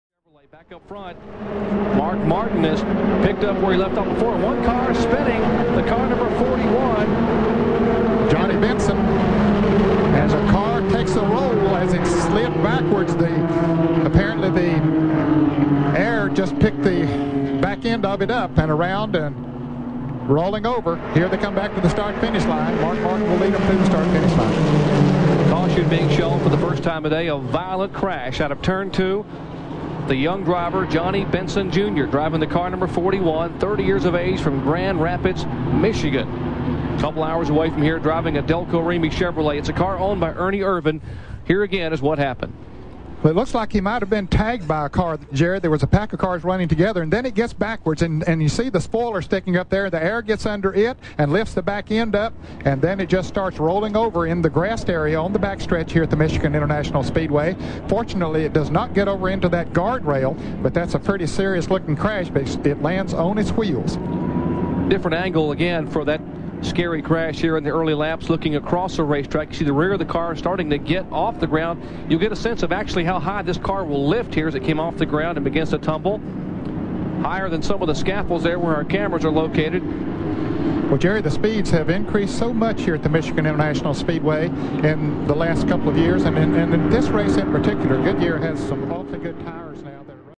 Johnny Benson Big Crash 1993 Detroit Gasket 200
NASCAR Busch Grand National Series Rd.17
Michigan International Speedway